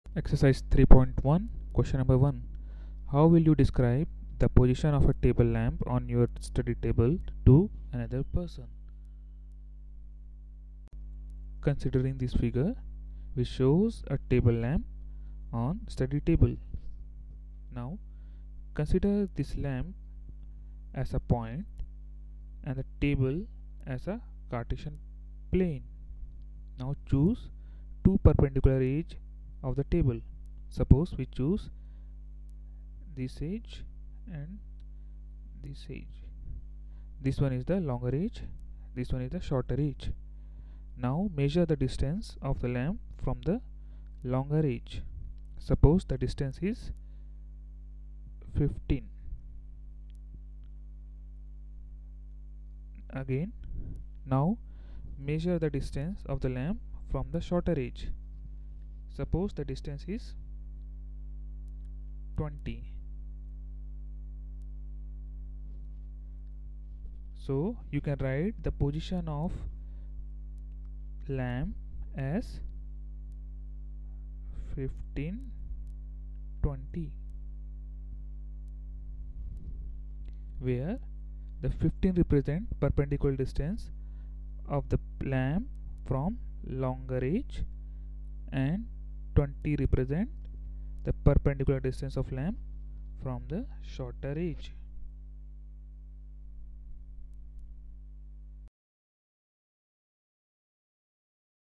CBSE Class-9 Maths NCERT solution - Coordinate Geometry - Exercise 3.1 - Problem 1
How will you describe the position of a table lamp on your study table to another
person?